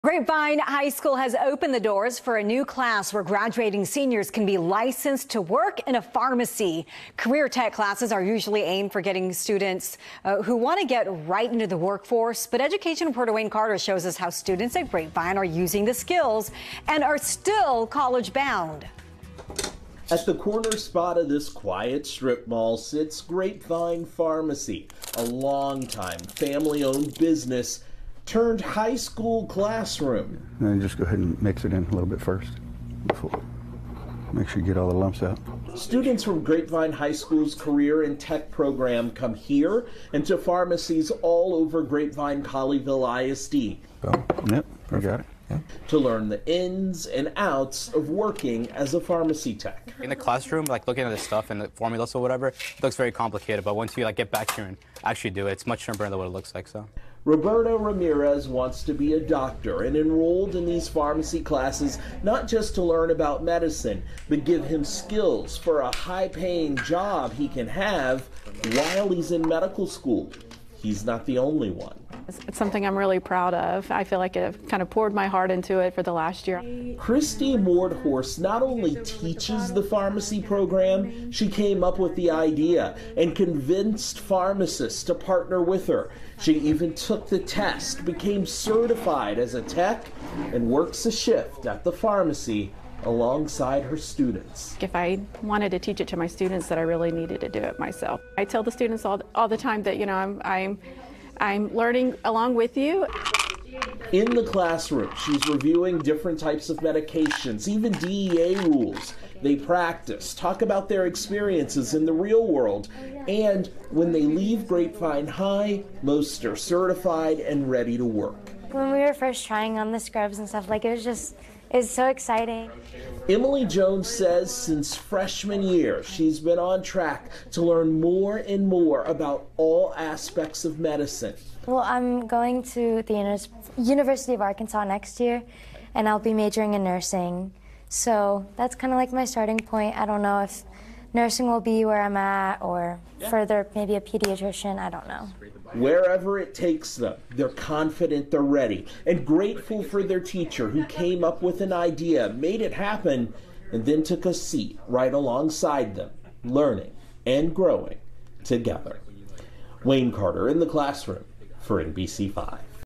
Grapevine pharmacy becomes classroom for high school students
Grapevine High School has opened the doors for a new class where graduating seniors can be licensed to work in a pharmacy. NBC 5's Wayne Carter explains the students are gaining skills while they're still college bound.